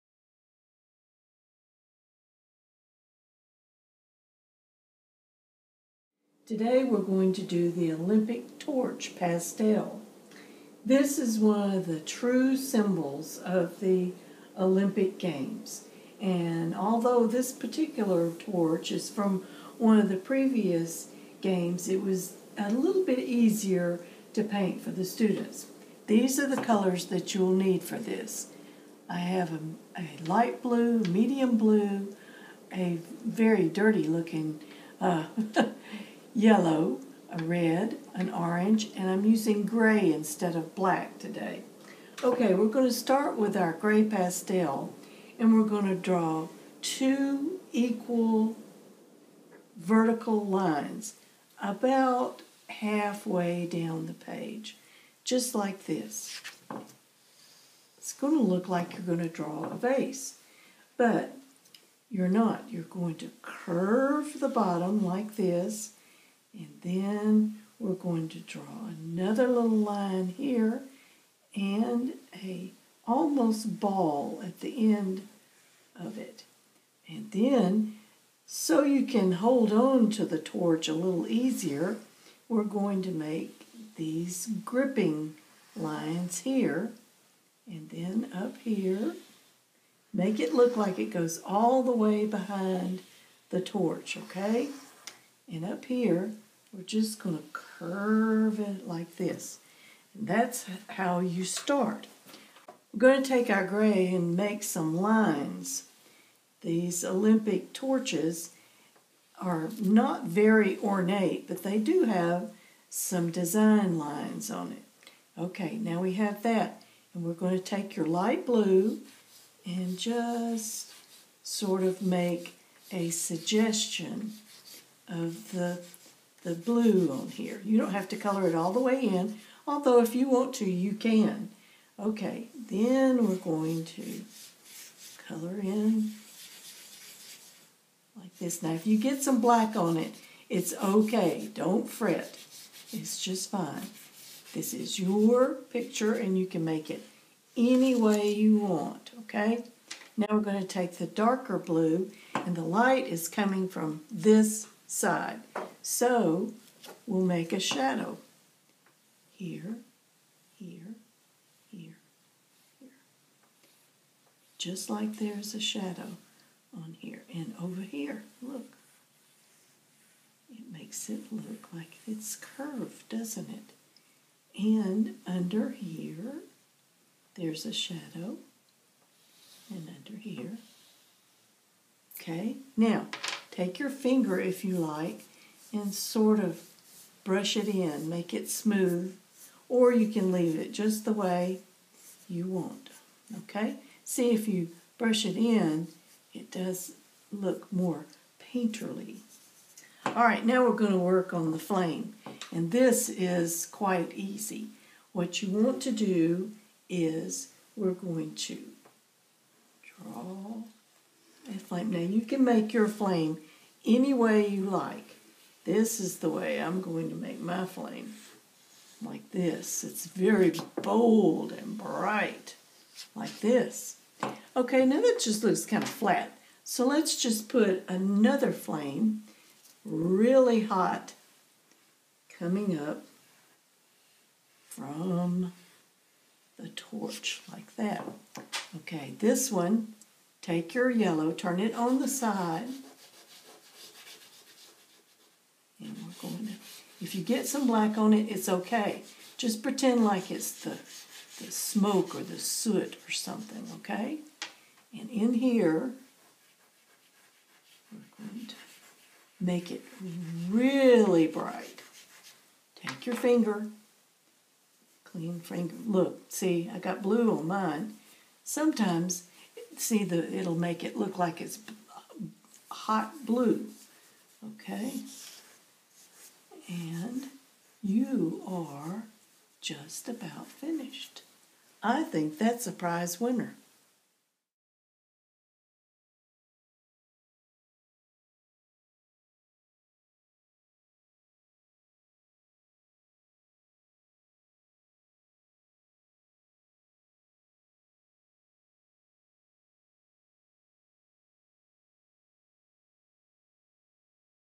How to Draw an Olympic Torch with Chalk Pastels
Nana shows us how to draw the Olympic Torch with chalk pastels. A companion to Nana's The Games video series. for even MORE video art lessons, You ARE an Artist Clubhouse membership and more!